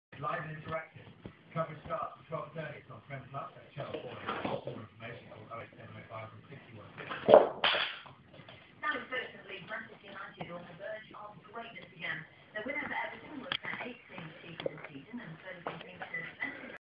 Scooby Doo - Falling Of My Leg
Scooby Doo my Mini Pinscher trying to climb up and say hello.